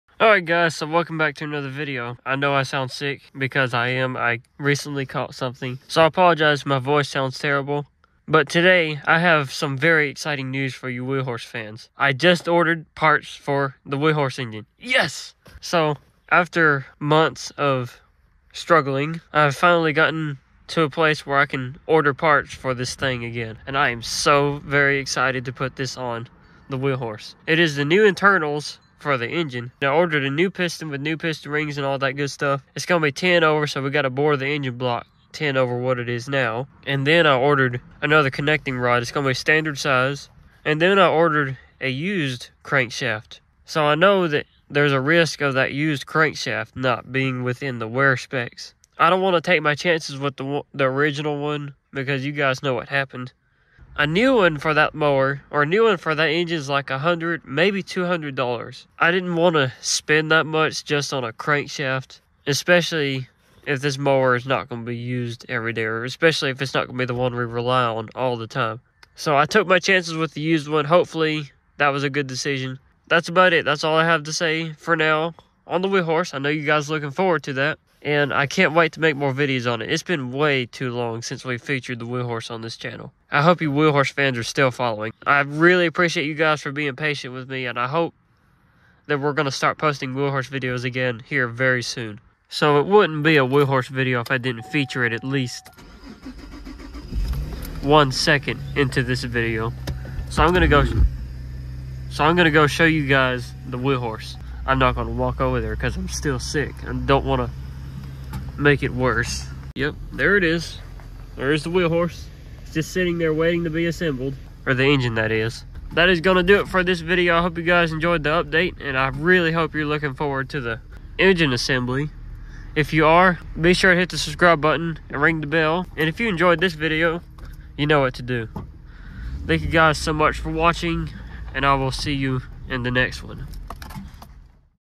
Ordered new parts for the Wheel Horse FINALLY!!
It's about time this happened!  I have finally ordered the parts needed to get the wheel horse fixed up again!  I appreciate you guys for being patient with me.  It looks like there will be a new wheel horse video soon.